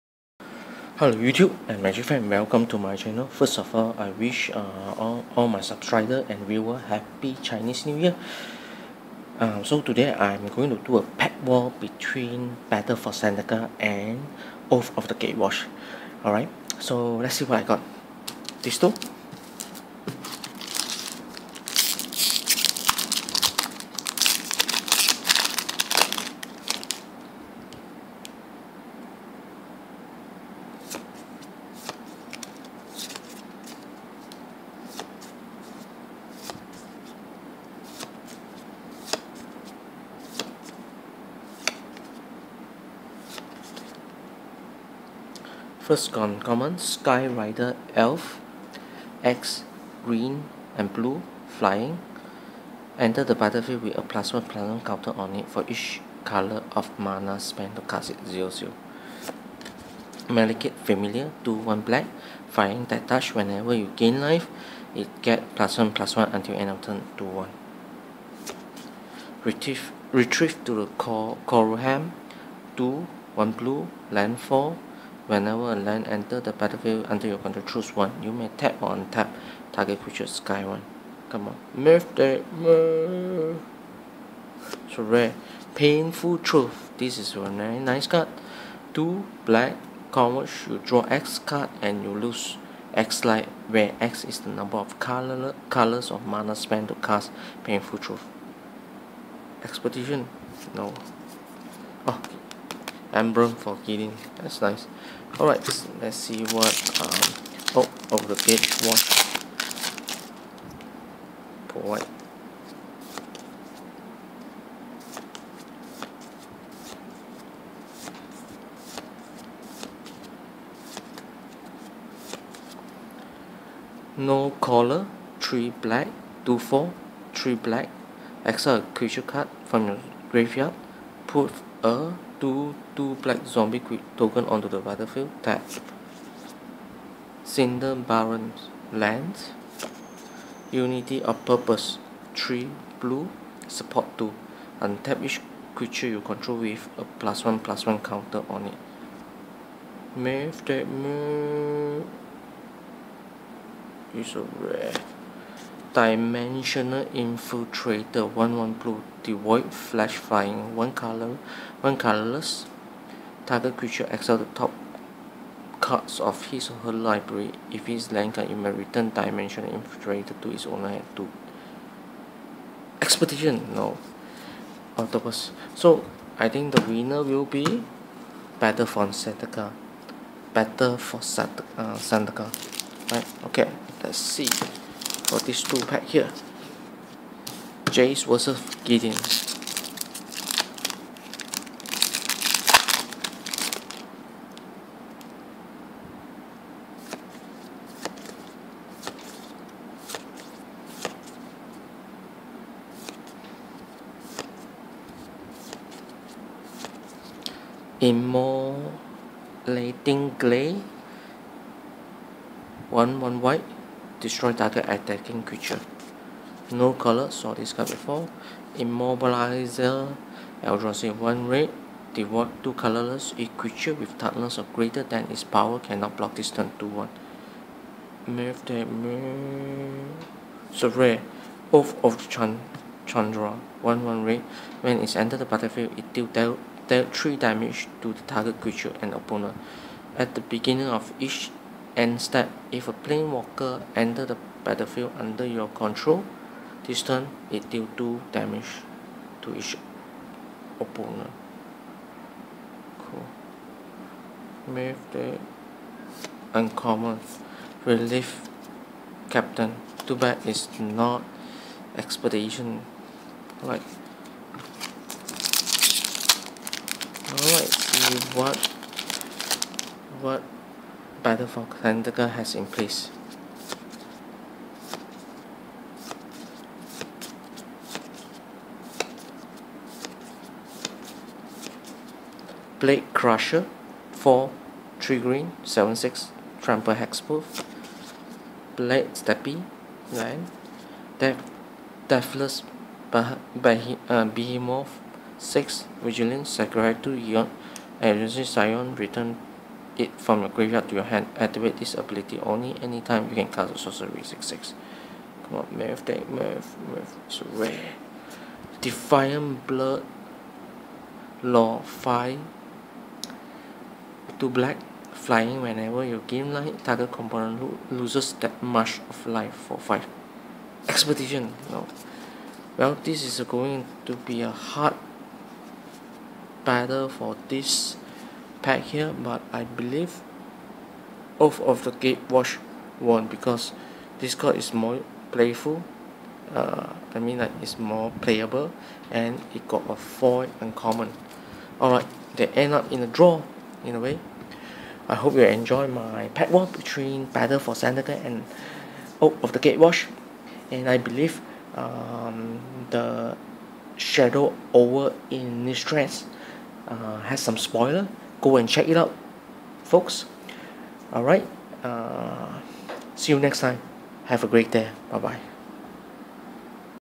MTG: Battle for Zendikar VS Oath of the Gatewatch Pack War! Happy Chinese New Year 2016!!
MTG Pack war between Battle for Zendikar vs Oath of the Gatewatch